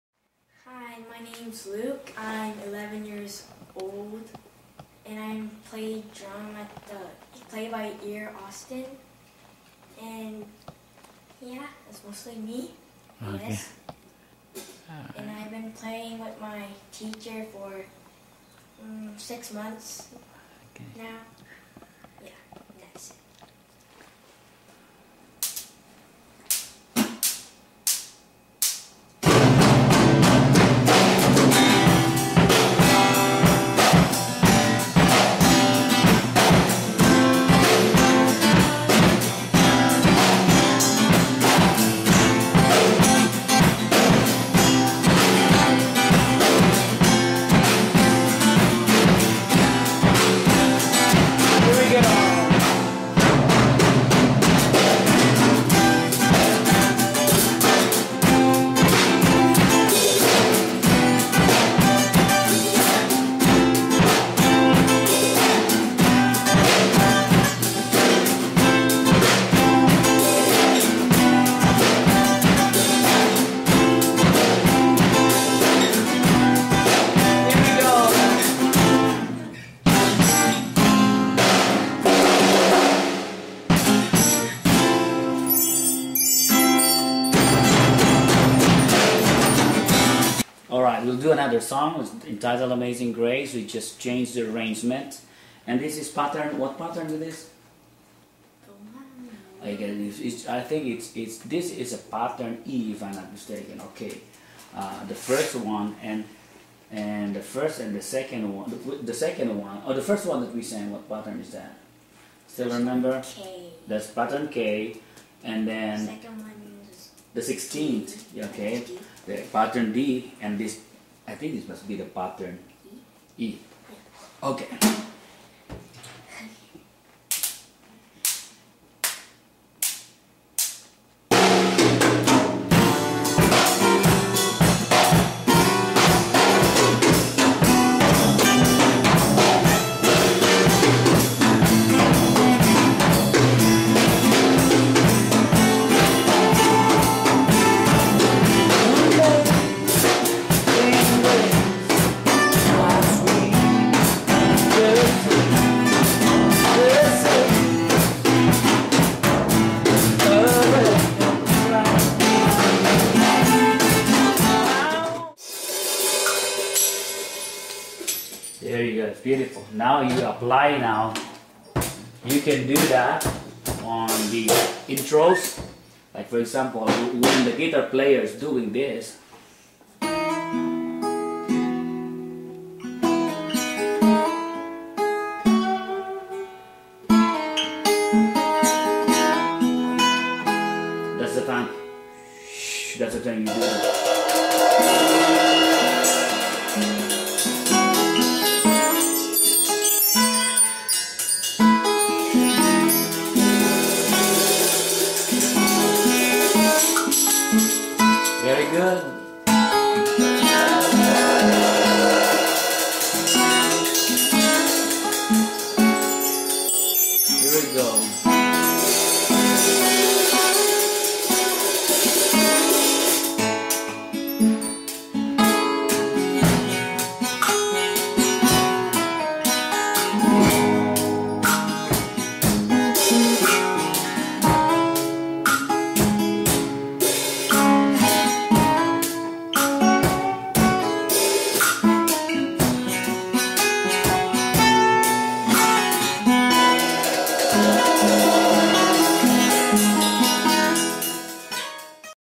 Luke Park
11 year old drummer soon will play with me for my gigs . . .